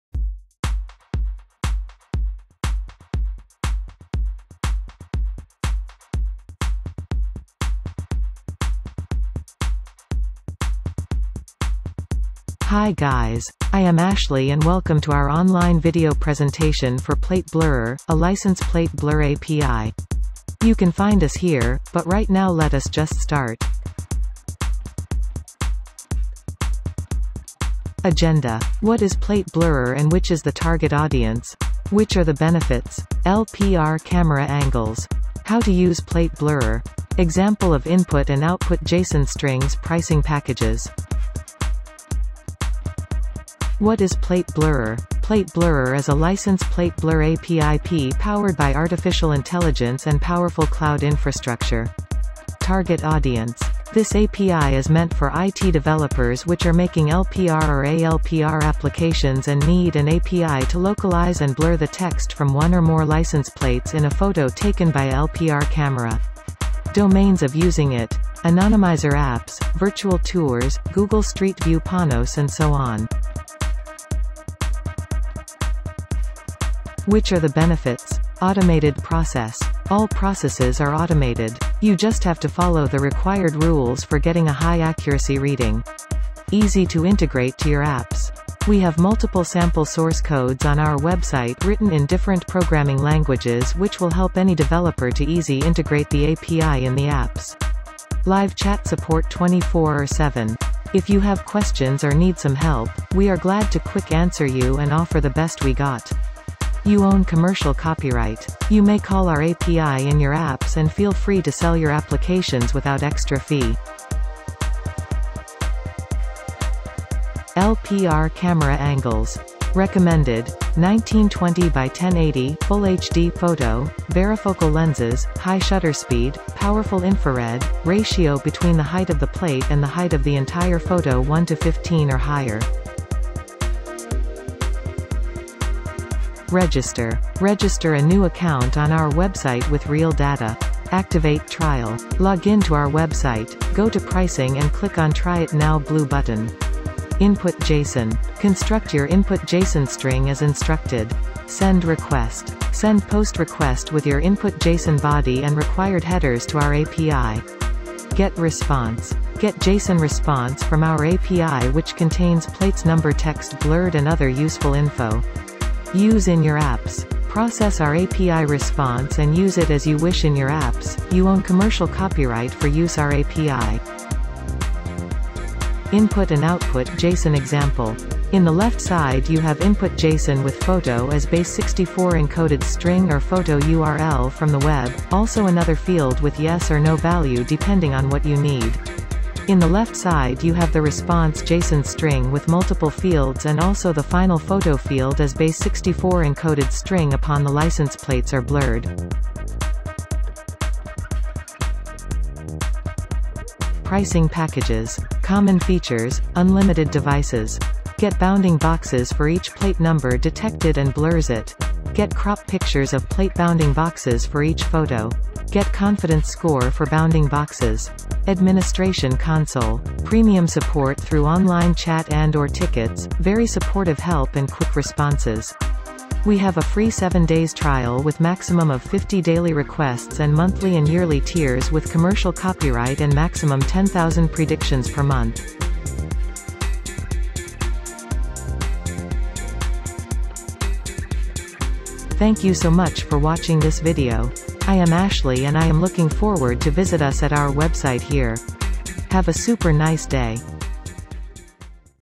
License Plate Blur API
License Plate Blur API is used to blur license plate numbers of the vehicles from a still photo. It is based on AI custom model on cloud infrastructure. It provides a base64 response string which is the initial photo upon which the license plates numbers are located and blured. You can find us here: